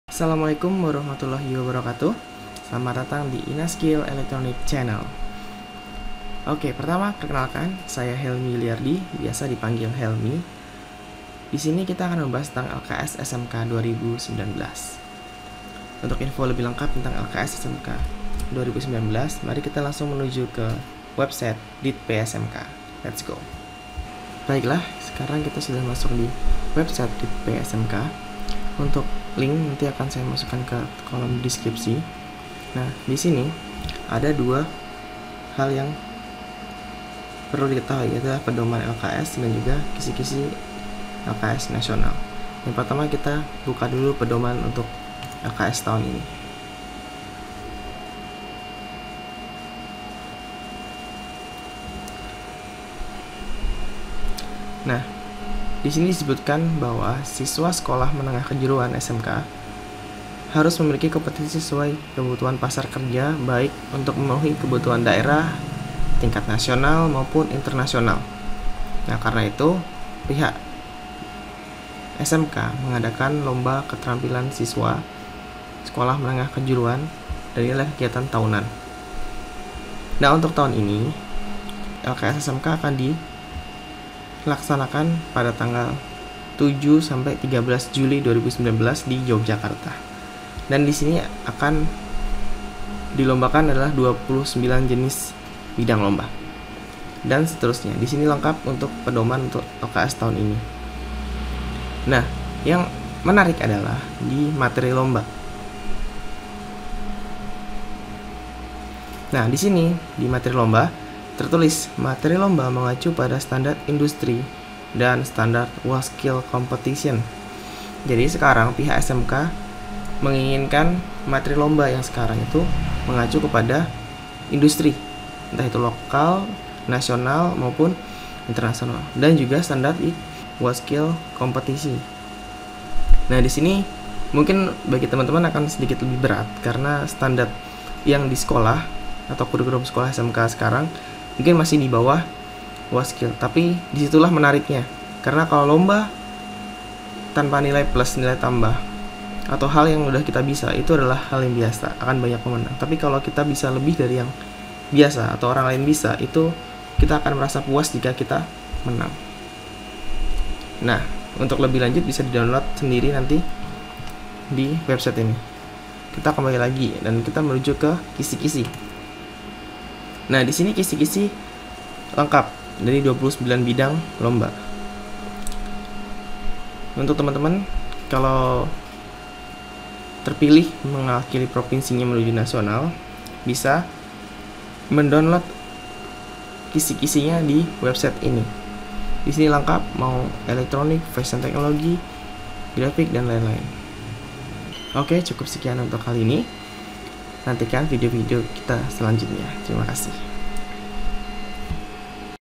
LKS 2019 Bagian 1 : Tentang LKS 2019
Tutorial untuk persiapan LKS 2019 Yogyakarta Bidang Electronics